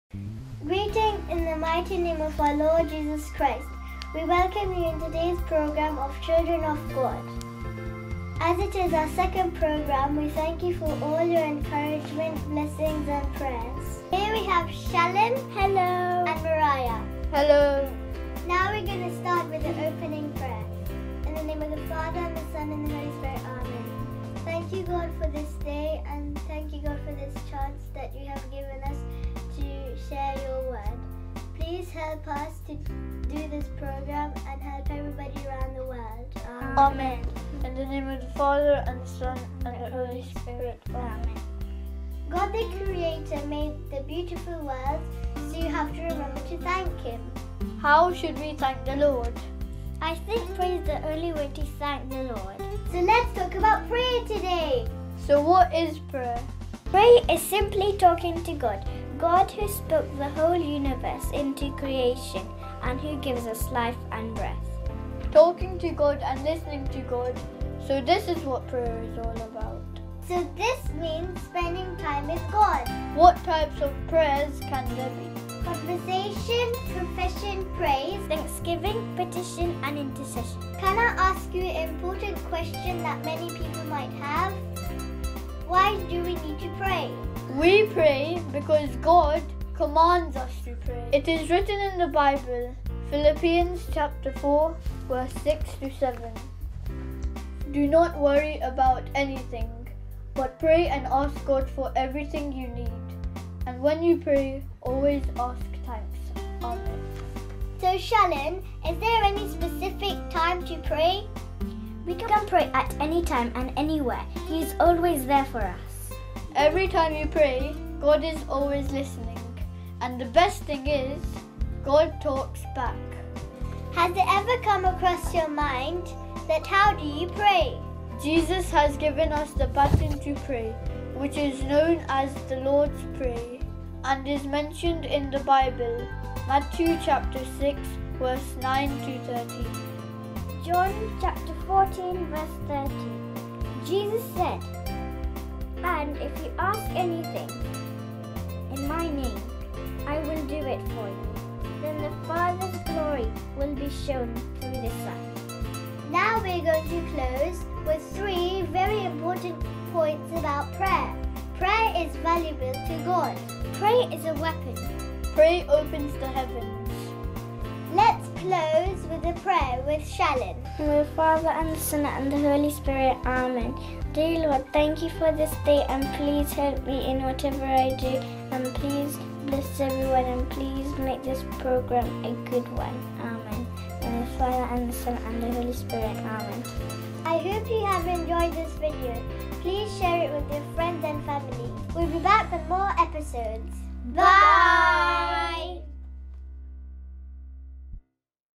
Children of God: Prayer Episode 2
What is Prayer? Why and How should we prayer?